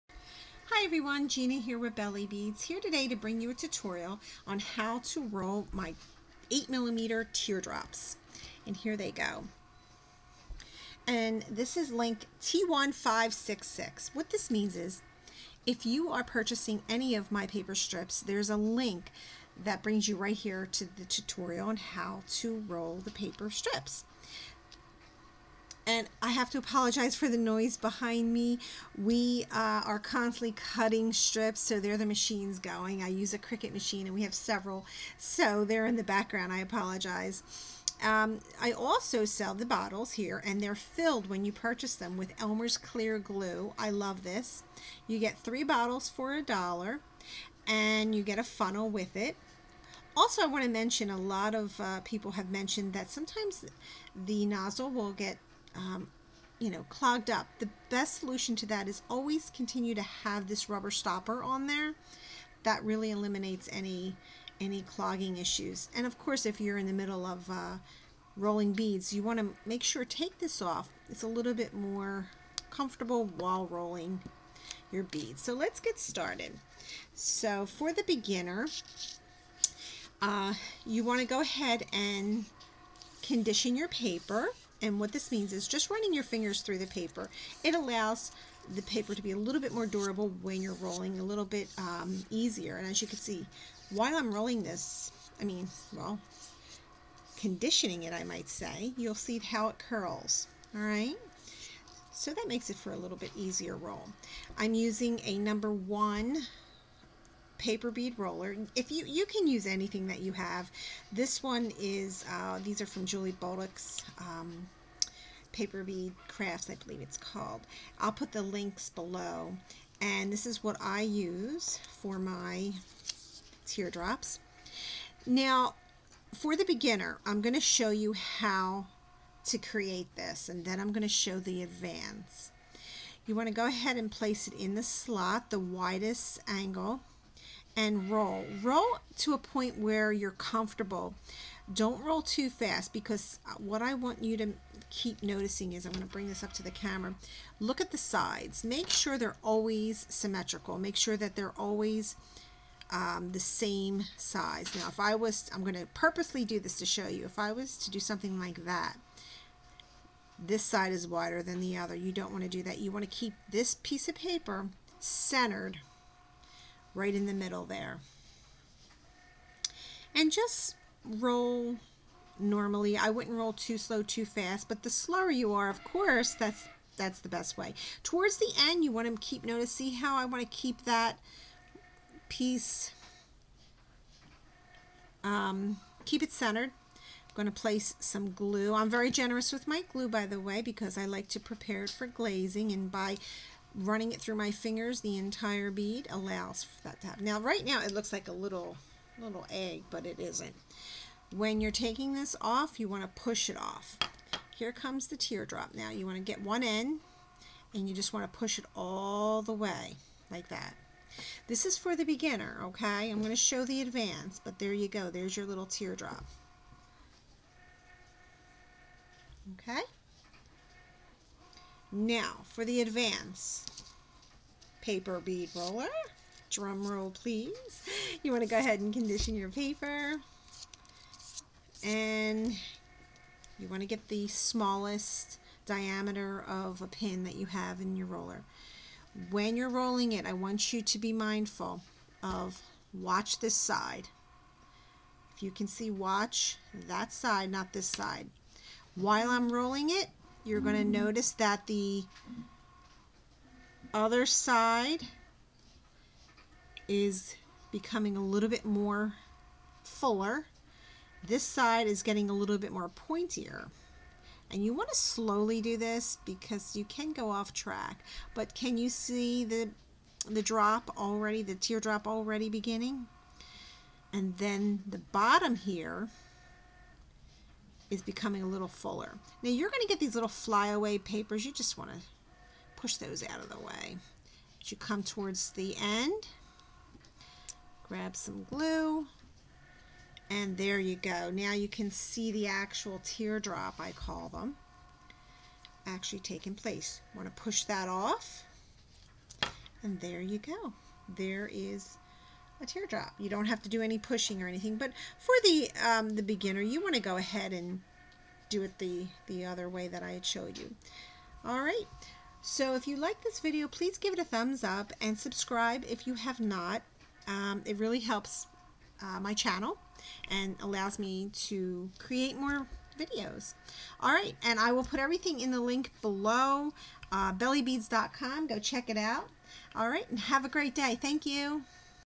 Teardrop Shape 8mm Paper bead-T1566
In this tutorial, I will show you how to roll my Teardrop 8mm Paper strips.

CORRECTION:  3/32 ROLLER IS USED NOT 5/32

You can purchase any size and shape of paper bead strips in my shop.
You can create beautiful jewelry or display them on picture frames, pottery vases, etc . Your imagination is endless once you begin with paperbeads!